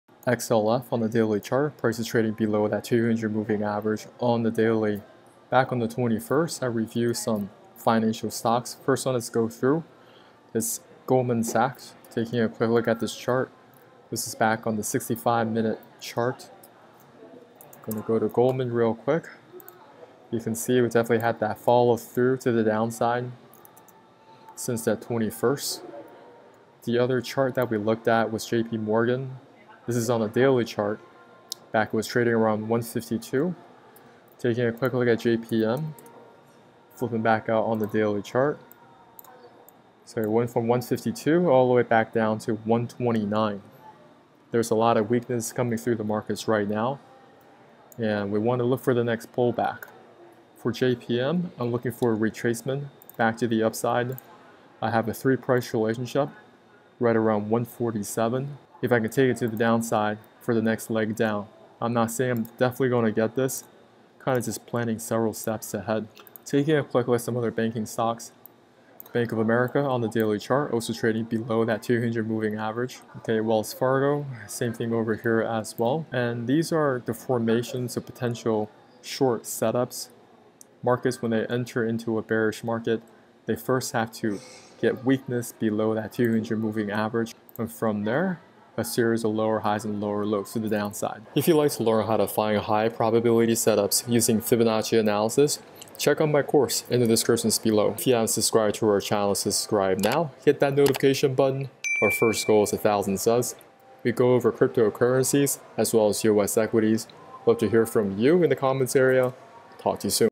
Financial SPDR Fund ($XLF) - More Downside? Review $GS $JPM BAC - Finances Showing Weakness 📉📉
💡 BitCoin: 3JQnmjFCMLAkw1yQEA4oVSXk7k4LsiEiWK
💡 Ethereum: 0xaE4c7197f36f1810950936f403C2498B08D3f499

- 𝗖𝗼𝗺𝗽𝗼𝘀𝘂𝗿𝗲
- 𝗖𝗹𝗮𝗿𝗶𝘁𝘆
- 𝗖𝗼𝗺𝗺𝗶𝘁𝗺𝗲𝗻𝘁